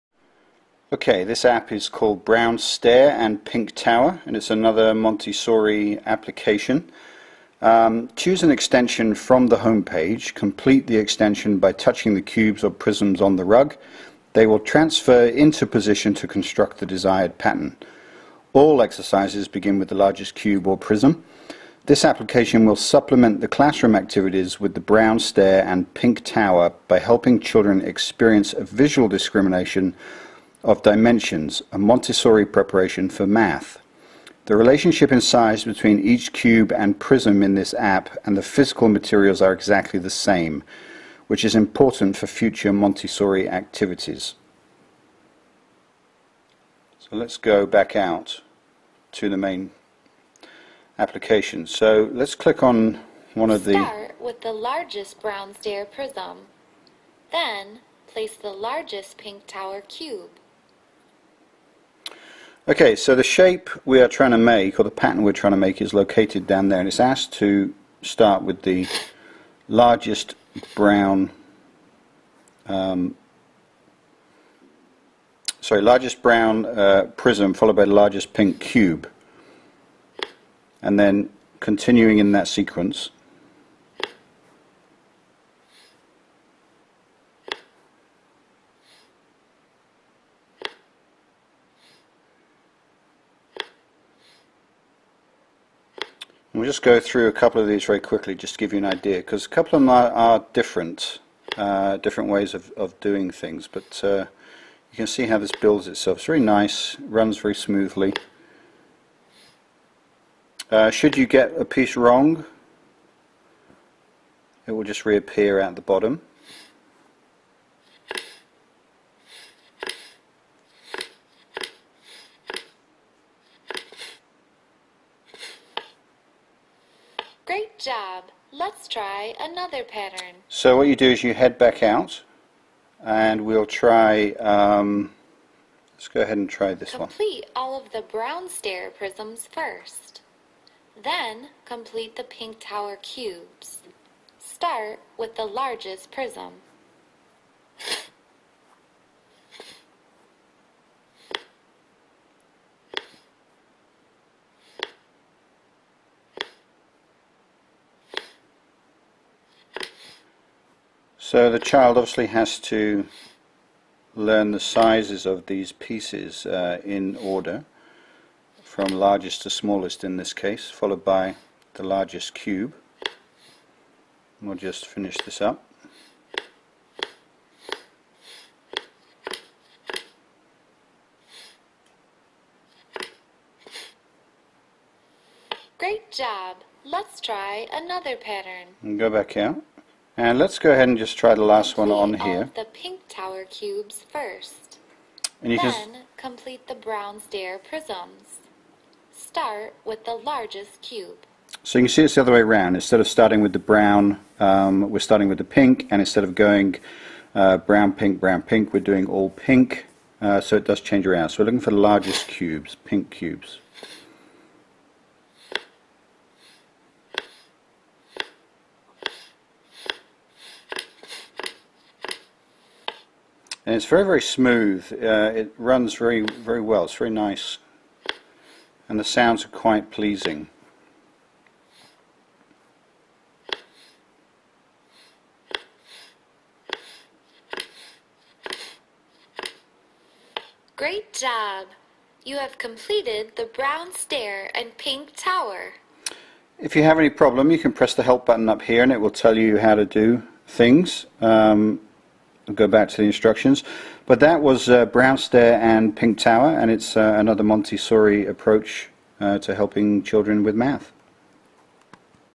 A Montessori Sensorial Exercise - Brown Stair & Pink Tower iPad App reviewed by a4cwsn
This app is the sequel to the Pink Tower application and contains more challenging extensions, giving children a visual discrimination of dimensions.

The dimensional relationship of the cubes and prisms in this app are exactly the same as physical Montessori materials and extensions like these are part of the introduction to math in the Montessori method.

This app was co-developed and approved by an AMI certified, Montessori teacher with over forty years experience teaching children!

For more iPad App reviews for children with special needs